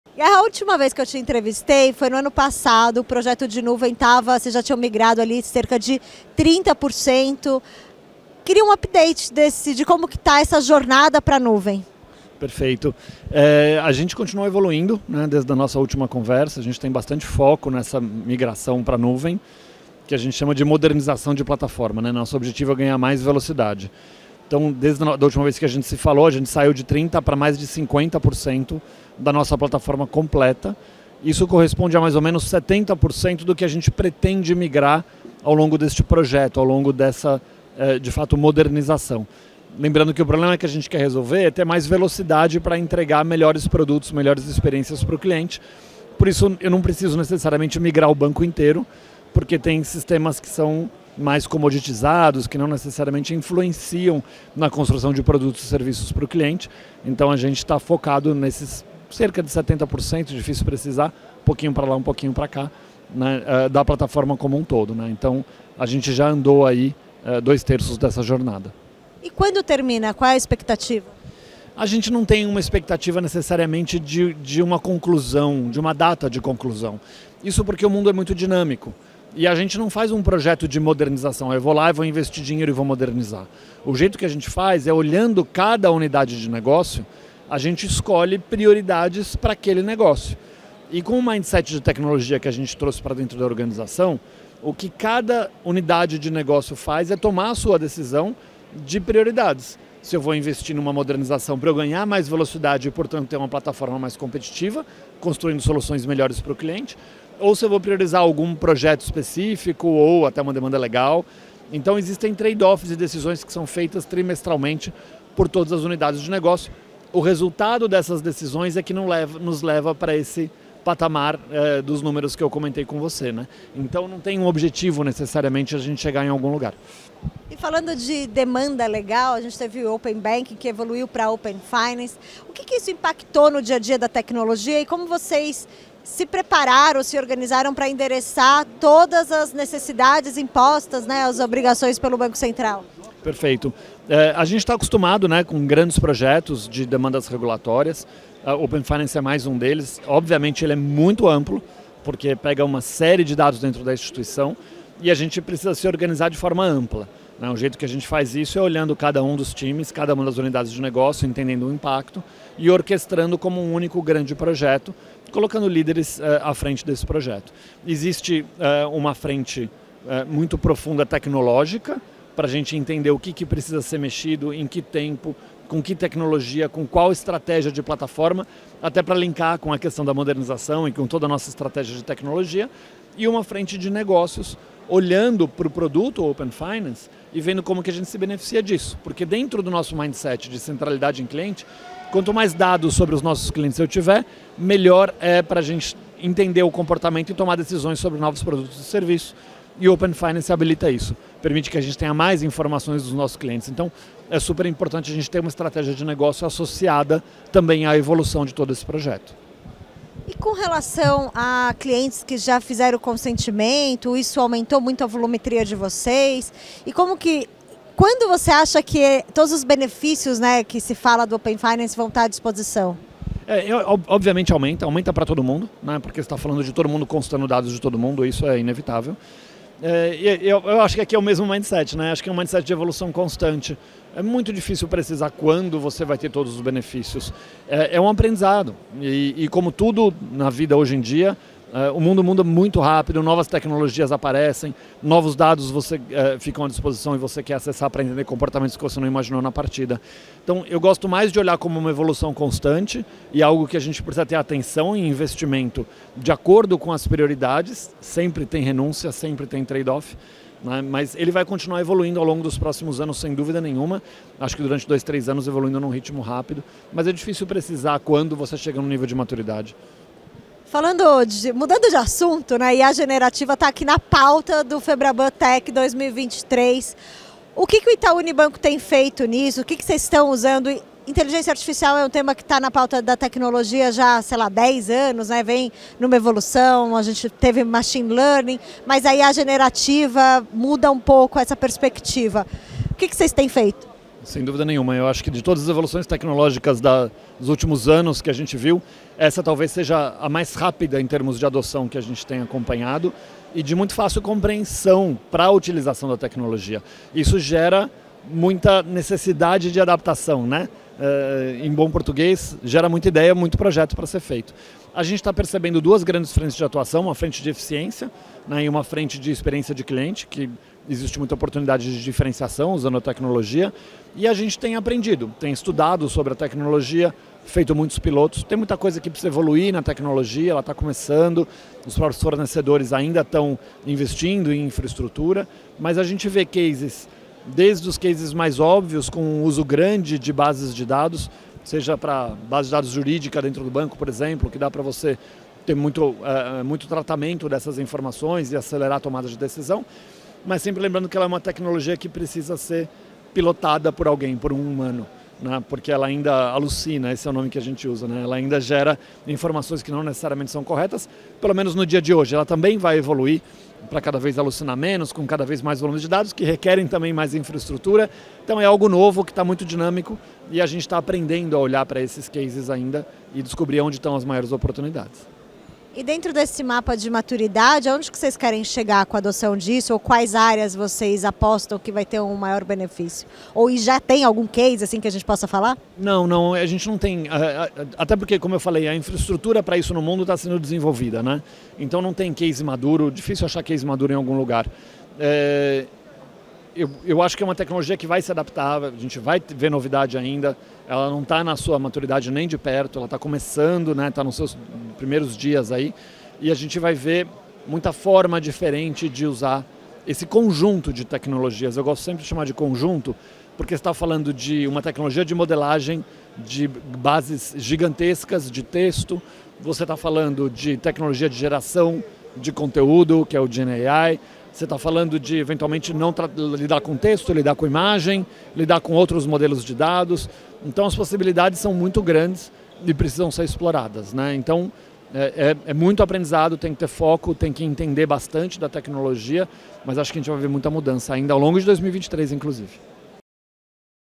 Guerra, do Itaú: IA generativa ainda alucina e precisa ser pilotada pelo humano
CIO do Itaú Unibanco, Ricardo Guerra, diz, porém, que a Inteligência Artificial generativa é a maior disrupção da tecnologia nos últimos tempos.  Executivo fala ainda que o Open Finance exige uma orquestração entre Tecnologia e Negócios.